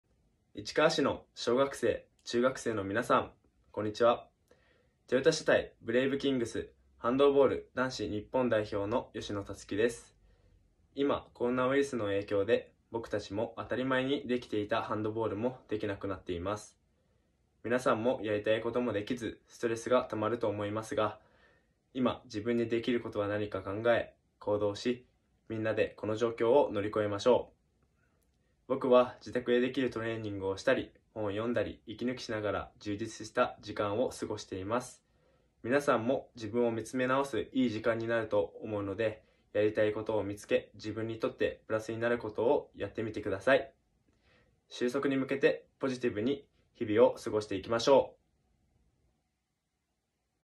吉野 樹 選手からのメッセージ
トヨタ車体男子ハンドボール部「ブレイブキングス」所属
市川市内高校出身